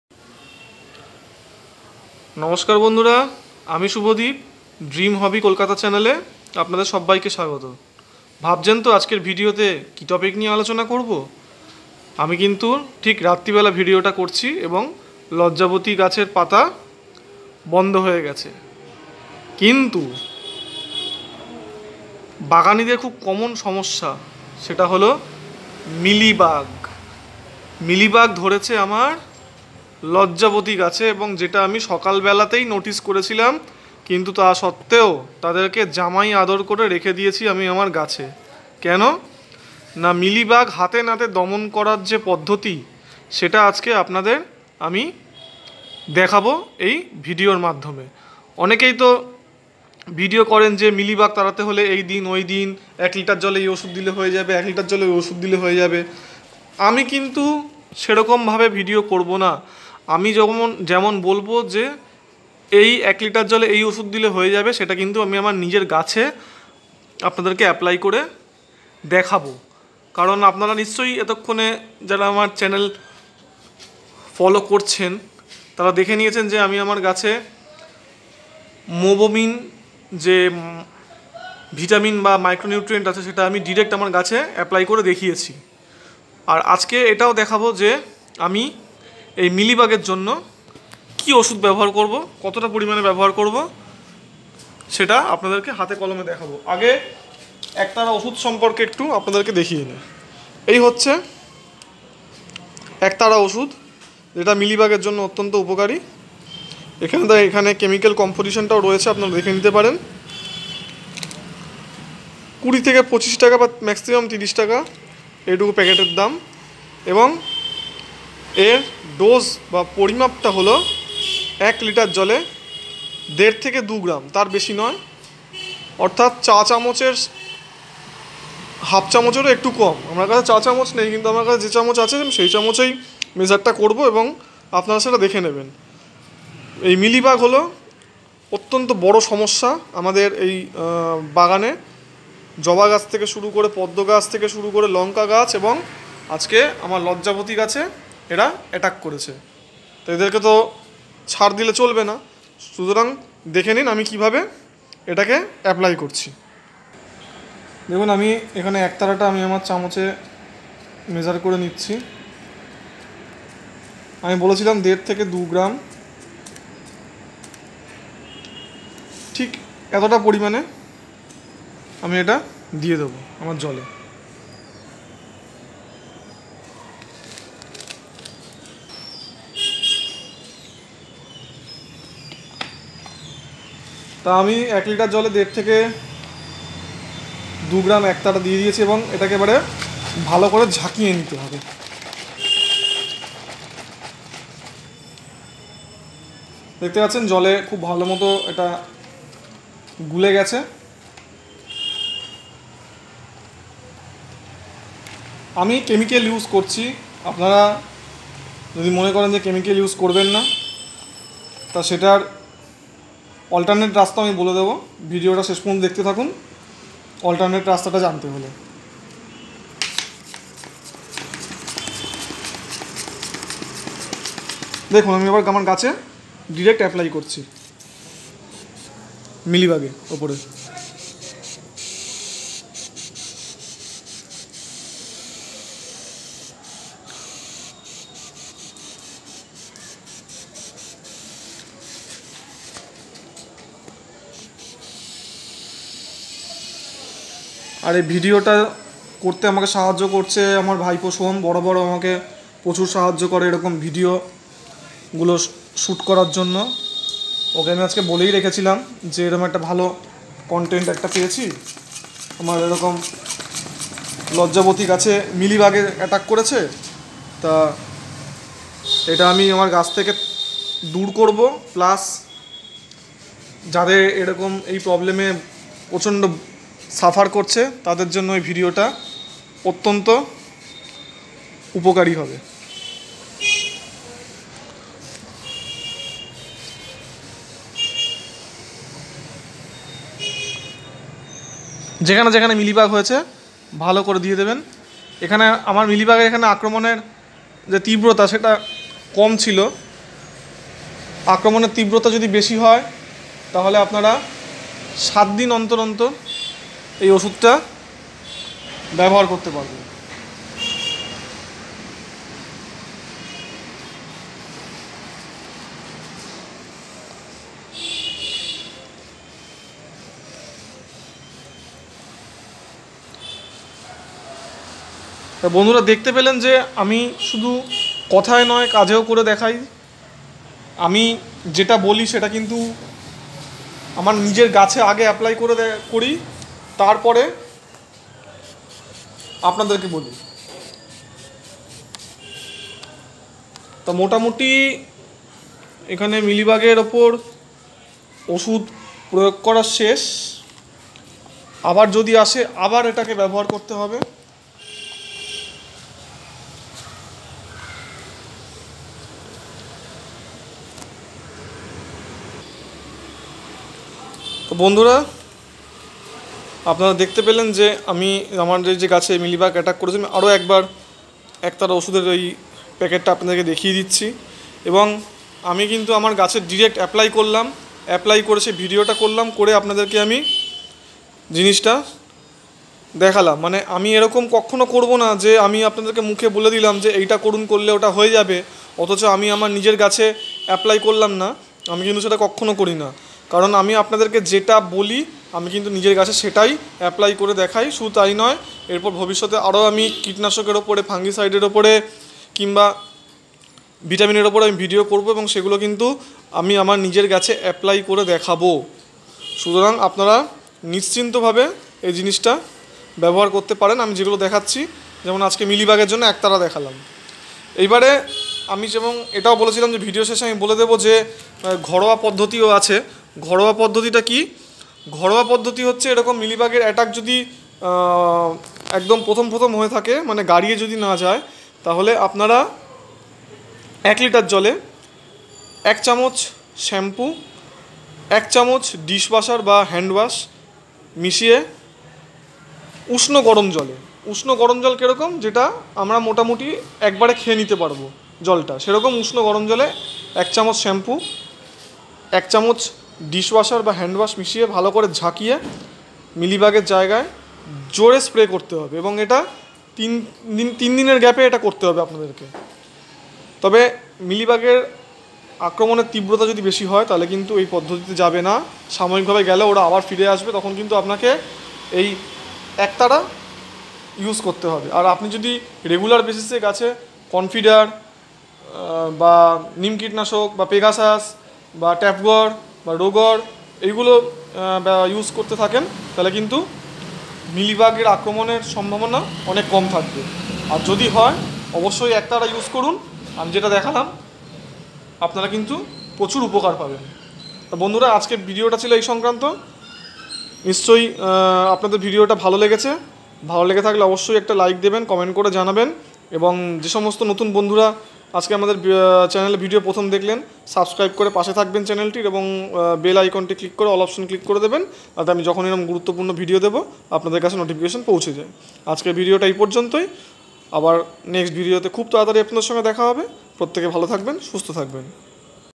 মিলিবাগ তাড়ানোর সব থেকে ভালো কীটনাশক এর ব্যবহার জেনে নিন।
প্রত্যেক বাগানিদের খুব কমন একটা সমস্যা মিলিবাগ। অর্থাৎ গাছের ডালে পাতায় সাদা তুলোর মত পোকা হয়ে থাকে, যা ধীরে ধীরে আপনার গাছ কে একদম শেষ করে দেবে। এই মিলিবাগ এর শরীরের ওপরে মোমের আস্তরণ থাকে যার দরুন সহজে এদের বিনাশ করা যায় না।

কিন্তু এই একতারা কীটনাশক আপনারা ব্যবহার করলে এই পোকার উপদ্রব অনেক কমে যাবে।

তবে মিলিবাগ না হলে এই কীটনাশক ব্যবহার করবেন না